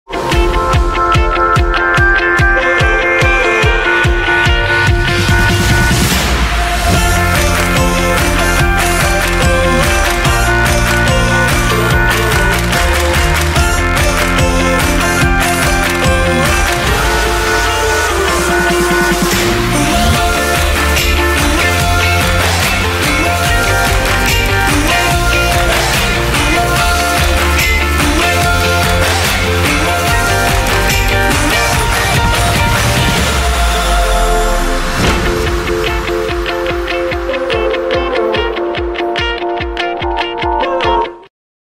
Welcome to Capital One City Parks Foundation SummerStage 2022!
Hey New York! Join us at Capital One City Parks Foundation SummerStage from June - Oct for nearly 90 free + benefit shows all summer long in NYC parks.

See you in the parks!

📧 Sign up for SummerStage emails for upcoming shows, livestreams and more